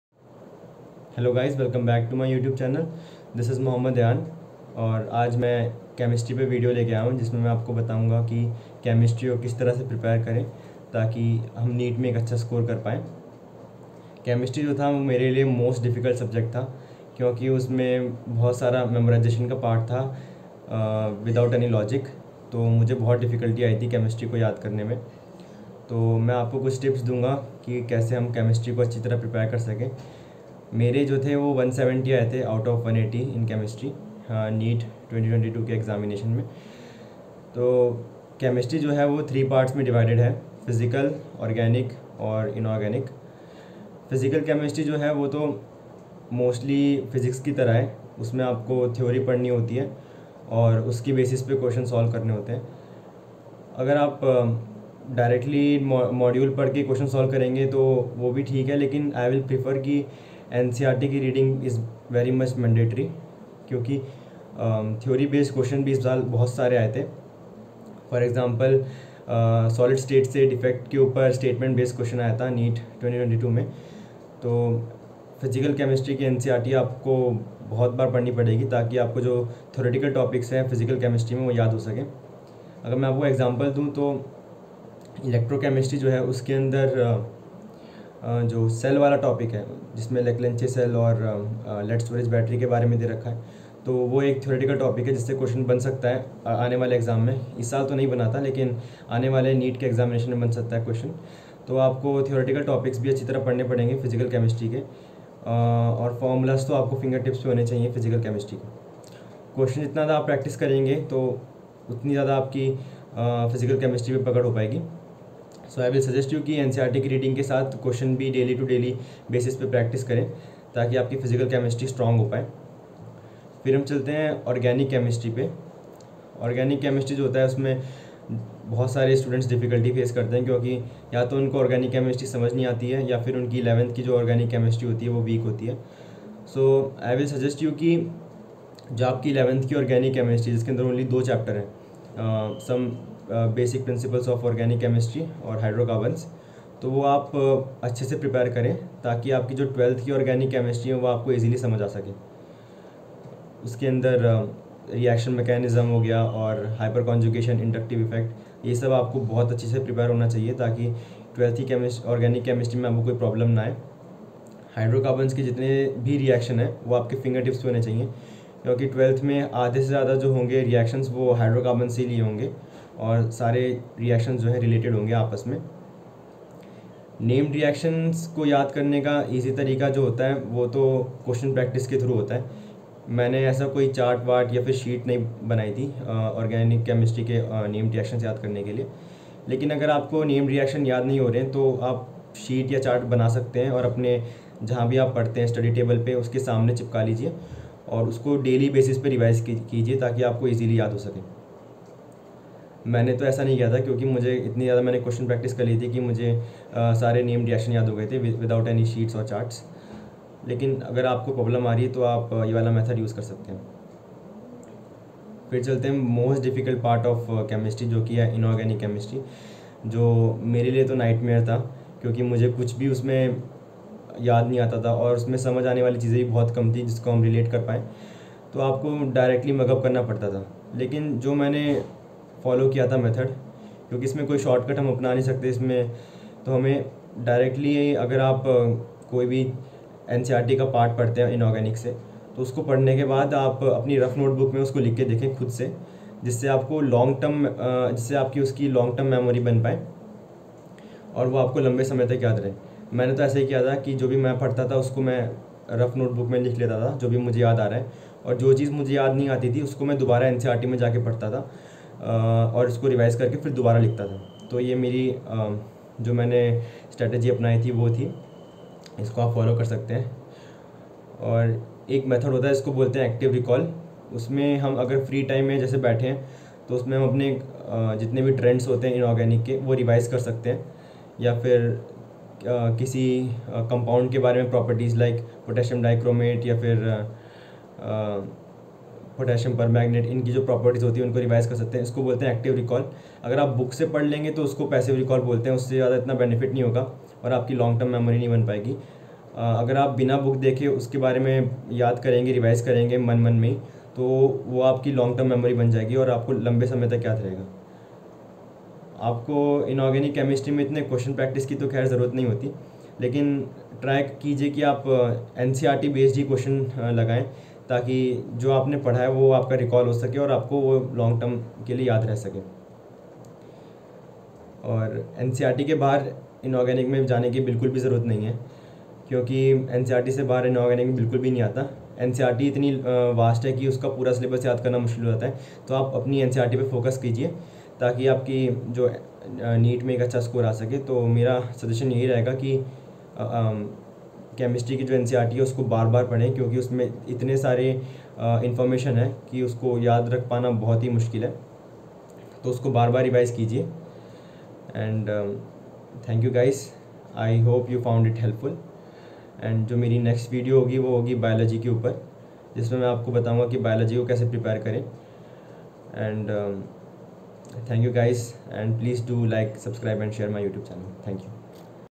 Best Strategy To Prepare Chemistry For NEET || Tips For Chemistry || AIR 187
Best Strategy To Prepare Chemistry For NEET || Tips For Chemistry || Chemistry Strategy For NEET || AIR 187

I had scored 170/180 in chemistry in neet 2022